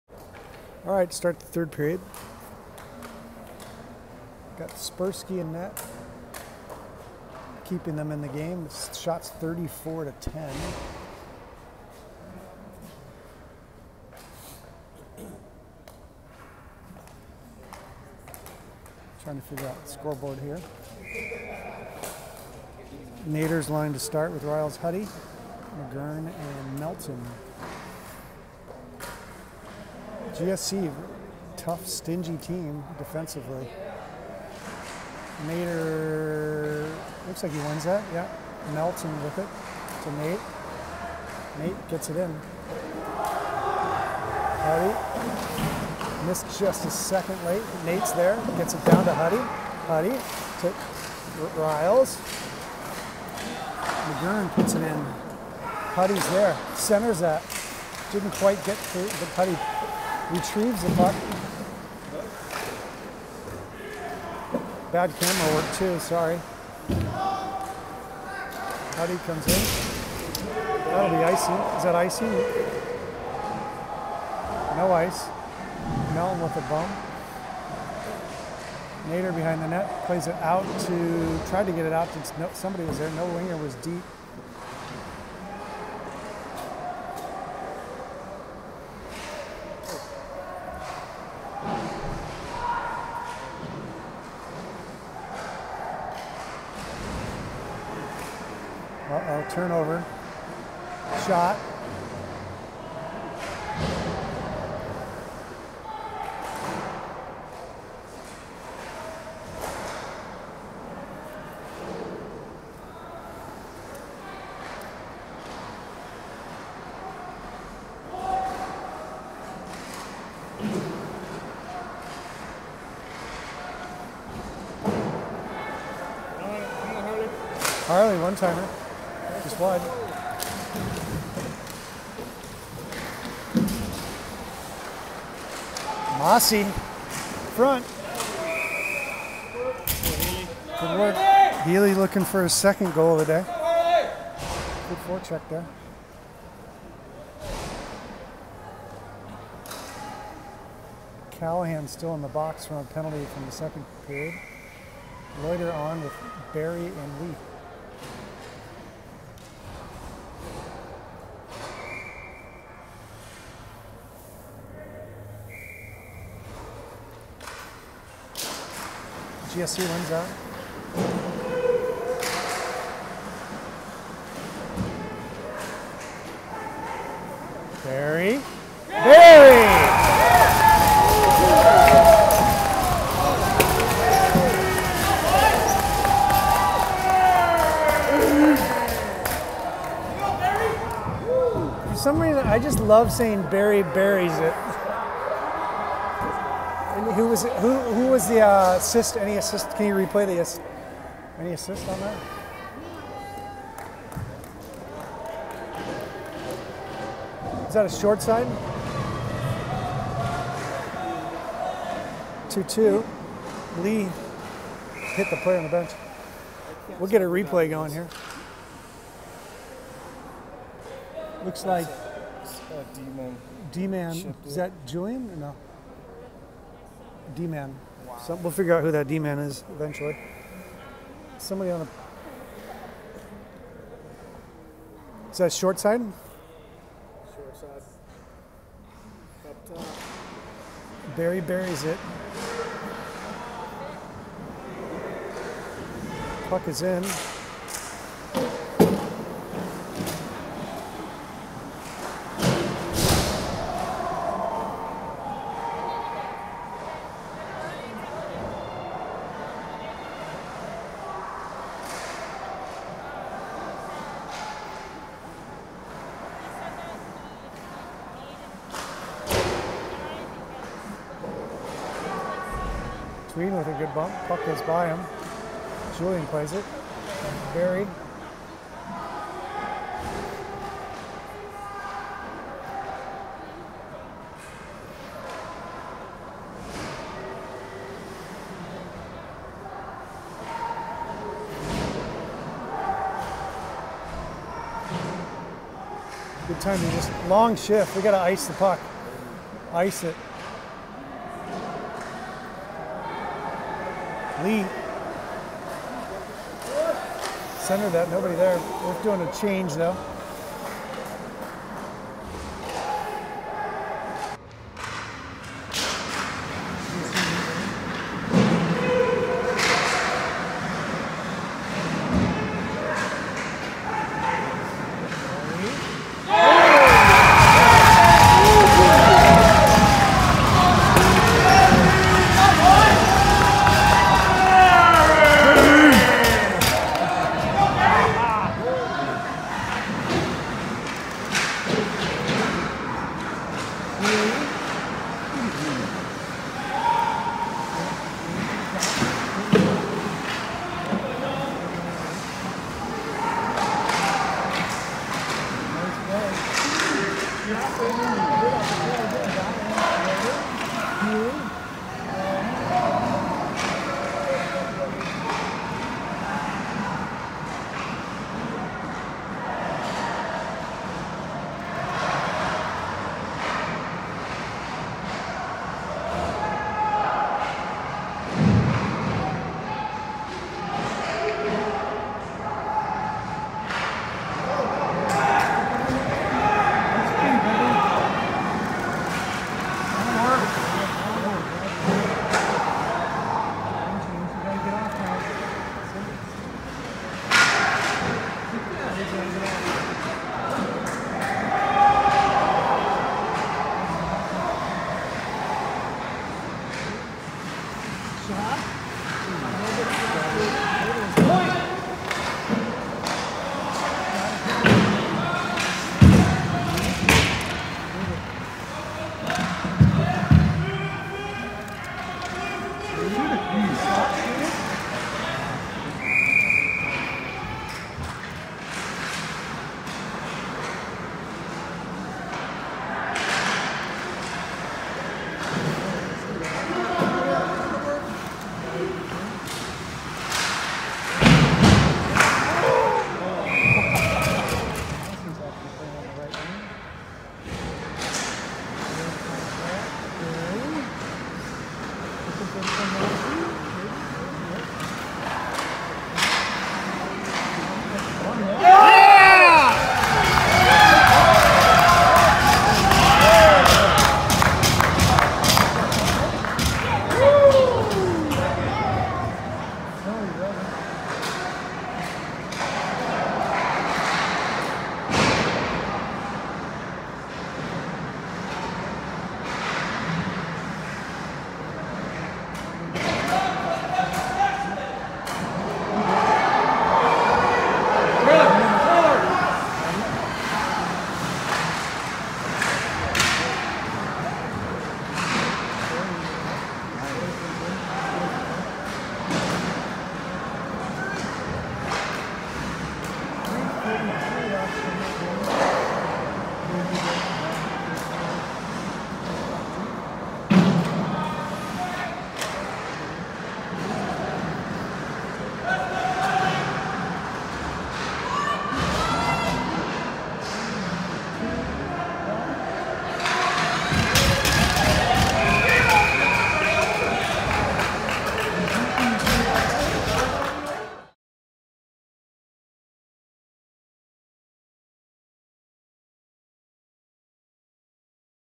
(3rd Period) Jr Ducks vs GSE U15 AAA - Vacaville
My video dropped off and lost everything in 3rd period.  These videos are from others at rink.